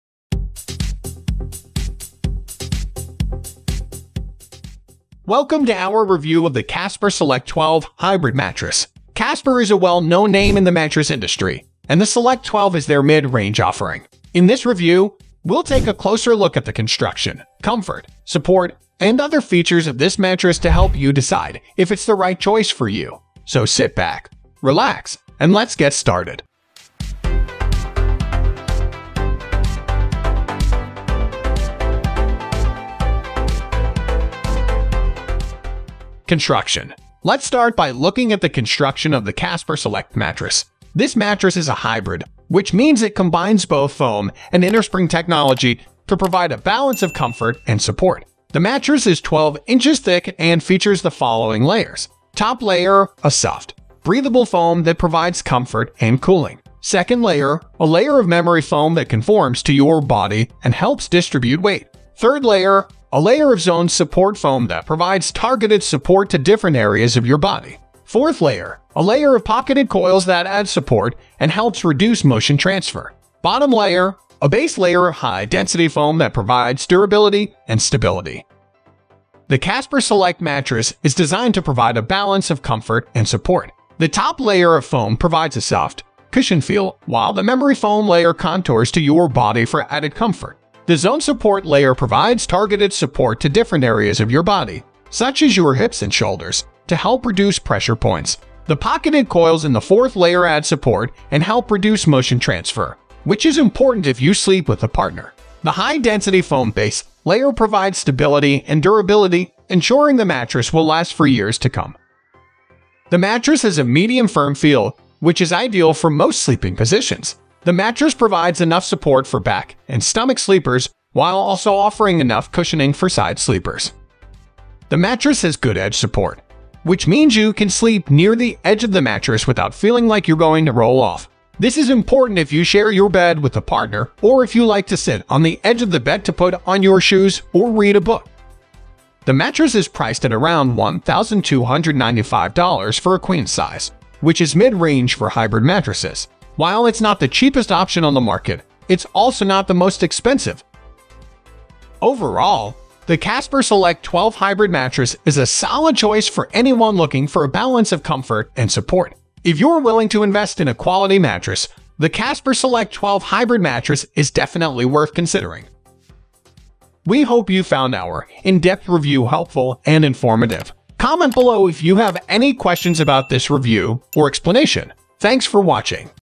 Casper Select 12 Hybrid Mattress Review - Is It Worth the Investment? [2024]
Welcome to our comprehensive review of the Casper Select 12 Hybrid Mattress! In this video, we'll provide you with an honest and unbiased assessment of the features, performance, and overall sleep experience offered by this popular mattress. Whether you're looking to upgrade your current mattress or searching for the perfect mattress for a good night's sleep, this review has got you covered.

Our expert reviewer will walk you through the key aspects of the Casper Select 12 Hybrid Mattress, including its innovative hybrid construction, advanced support system, pressure relief capabilities, and exceptional motion isolation properties. We'll also discuss the materials used, the mattress's durability, and how it performs for different sleeping positions.

But that's not all! We'll share real-life experiences from verified customers, highlighting their satisfaction and comfort levels. You'll also discover important information about Casper's warranty, trial period, and customer service to help you make an informed buying decision.

If you're searching for the perfect mattress that combines the support of coils with the plush comfort of foam, the Casper Select 12 Hybrid Mattress might just be what you're looking for. It's suitable for various body types and sleep preferences, making it an excellent option for couples, back sleepers, side sleepers, and even stomach sleepers.

So, join us as we dive deep into the features and benefits of the Casper Select 12 Hybrid Mattress. Make the right choice for your sleep needs with our expert guidance!